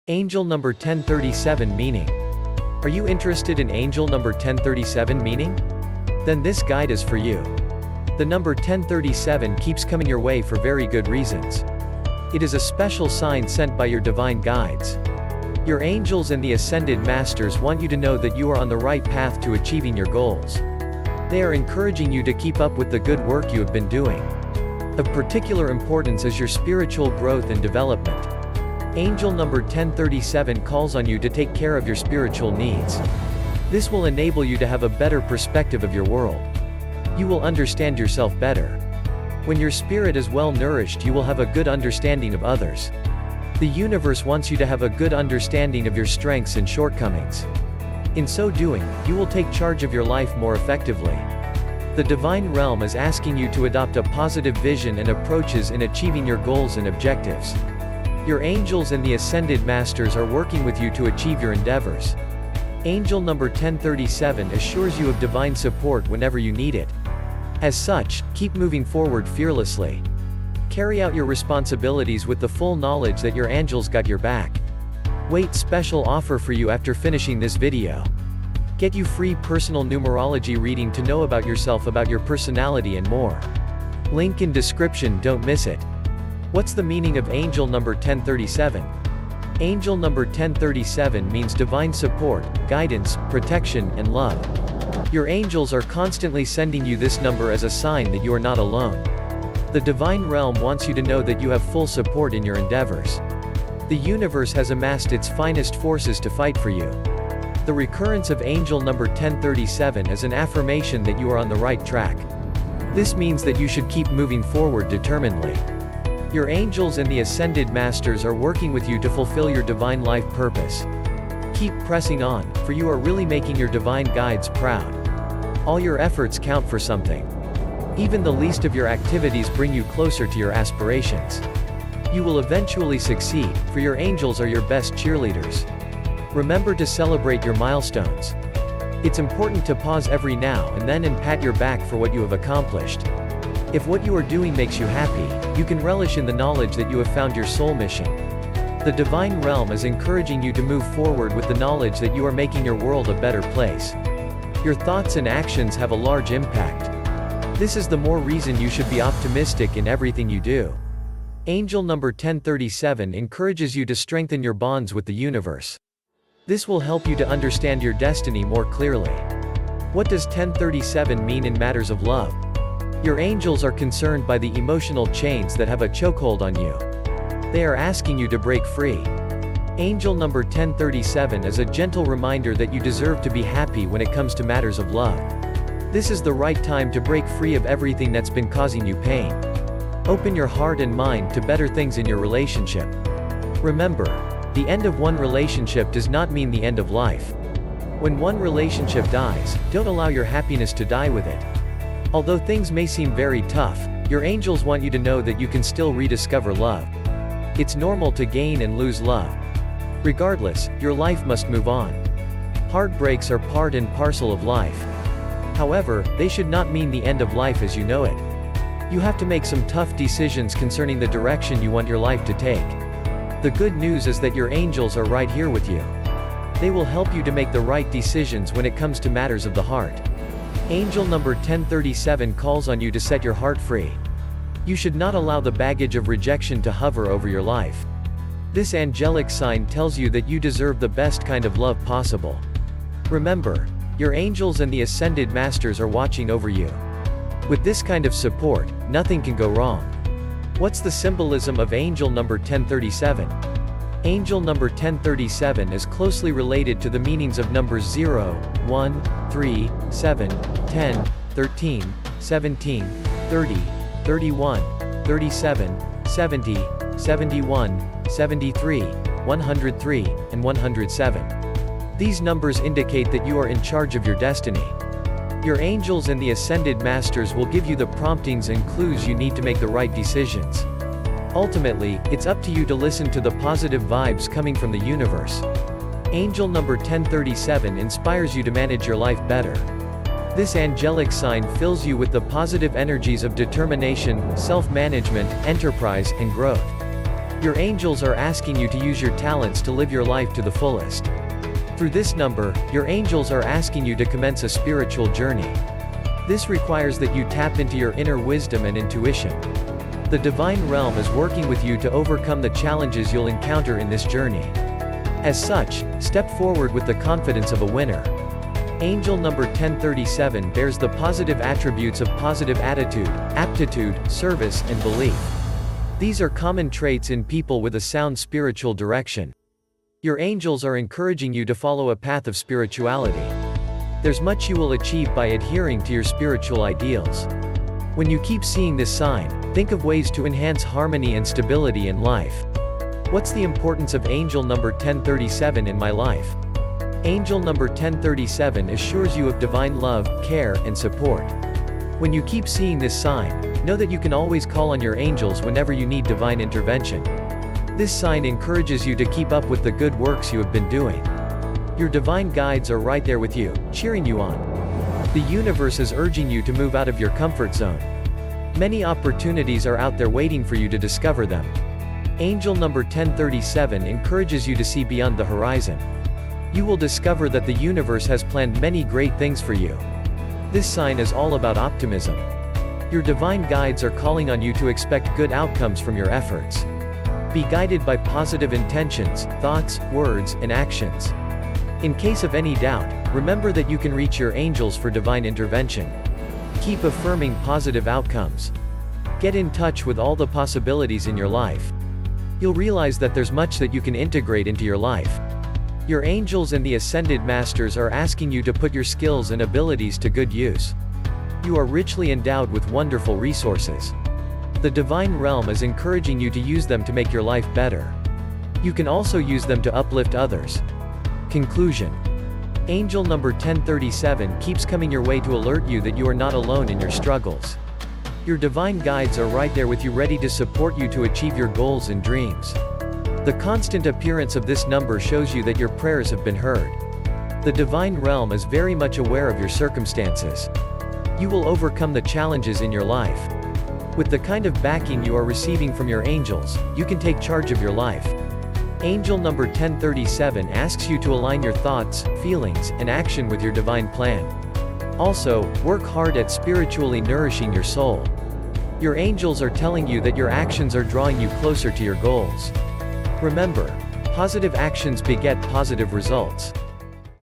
Angel Number 1037 Meaning: Your Life Will Change Forever!
In this video, we dive deep into the angel number 1037 meaning, uncovering the powerful messages hidden within this unique combination of digits. As you explore the spiritual significance of this number, you'll discover how it encourages you to trust your intuition and align your actions with your highest purpose. The angel number 1037 meaning also signifies the presence of divine guidance in your life, urging you to embrace positive changes and follow the path that resonates with your soul's true calling. Whether you're facing challenges or seeking clarity, understanding the angel number 1037 meaning can provide you with the insight and reassurance you need to move forward with confidence. Join us on Numerology Host as we break down the symbolism and spiritual wisdom behind this powerful angelic message.